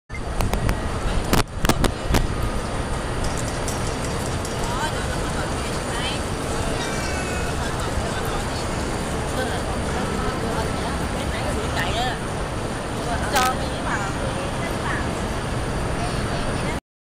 Microsoft Kin Two: Times Square - Standard Definition
Short nighttime video of New York's Times Square on May 4, 2010. Taken with the new Microsoft Kin Two cell phone for Verizon Wireless. Compare detail of this video with video from same spot taken with the high-definition setting. The new phone reaches stores May 13.